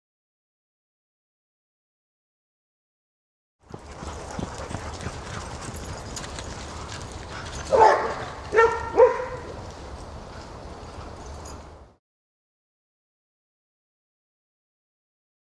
Dog Trots on trail
Gloria Cropper of Dog Trots offers off-leash exercise for your dog while you're at work.